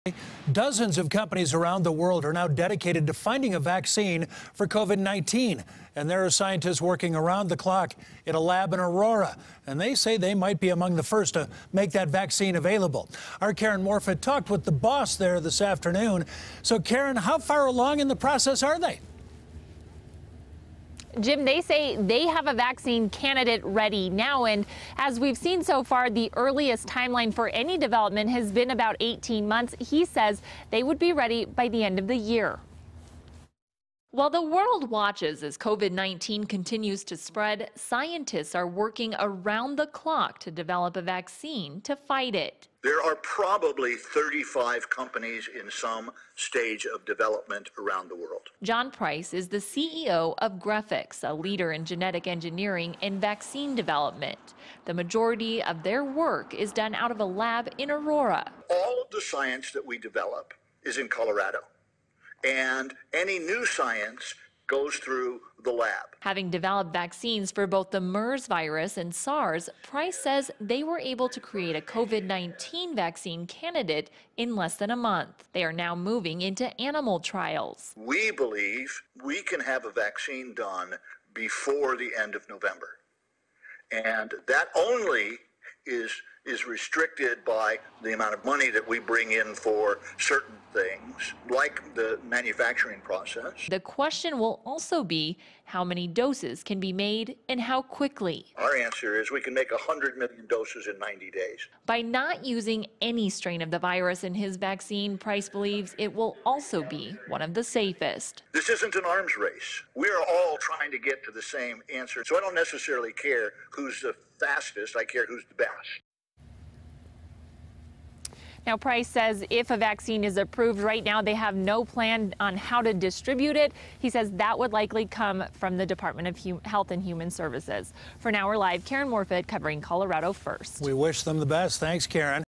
Researchers In Aurora Are Trying To Develop A Vaccine For Coronavirus
Dozens of companies around the world are working to develop a vaccine for COVID-19, and Colorado scientists are among those working around the clock.